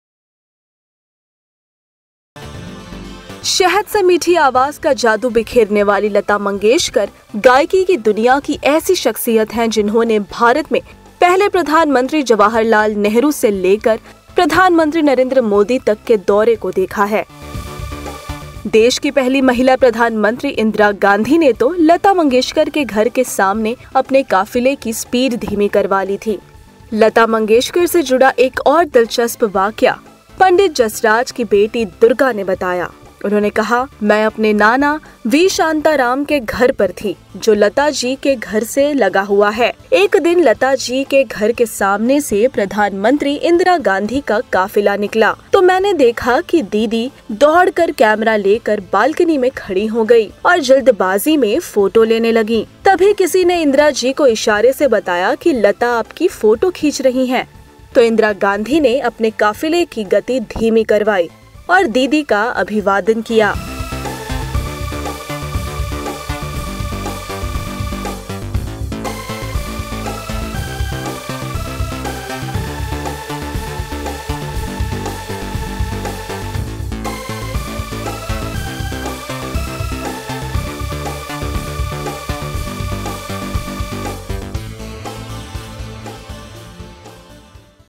जब Indira Gandhi ने Lata Mangeshkar के लिए अपना काफिला धीमा कर दिया
Former prime minister of India Indira Gandhi was fond of music and shared a beautiful bond with Lata Mangeshkar. Infact, there was one incidence where Indira Gandhi slowed down her convoy for Lata Mangeshkar. To know more about this interesting story watch the video. LataMangeshkar, IndiraGandhi, About Lehren